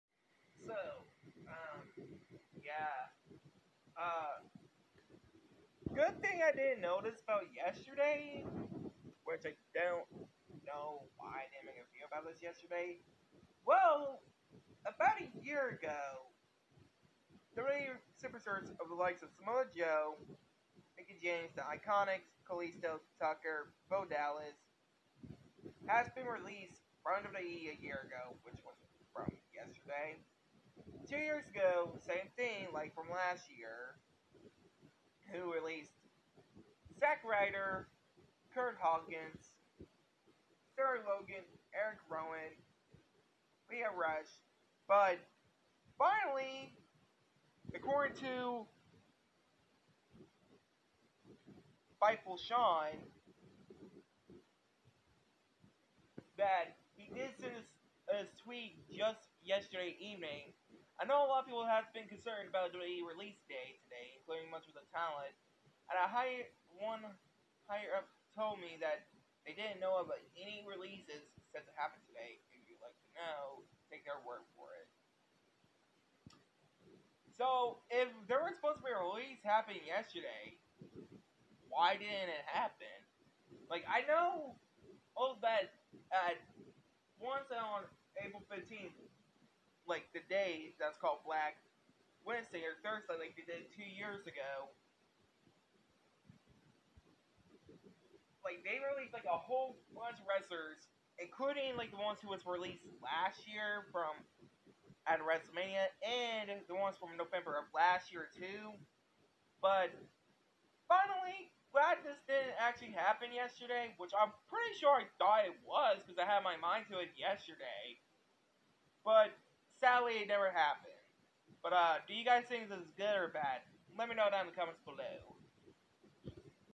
No WWE Releases From Yesterday
Arcording To Fightful Sean That Would WWE Release Talents But Luckily I'm Surprised That They Didn't Do WWE Releases Yesterday